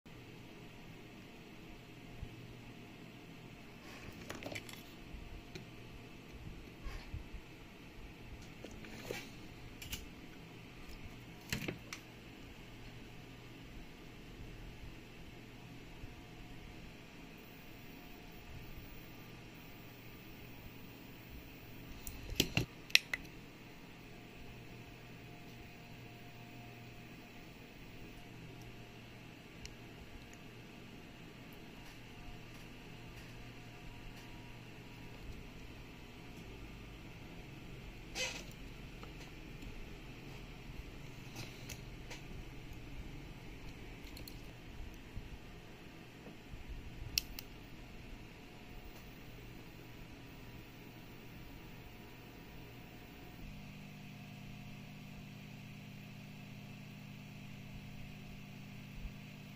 Homemade ESR-meter vs Multi-fonction TESTER TC-1. A DISASTER.
This tester is not able to read a ESR value correctly. If you want a real ESR meter, don't buy it.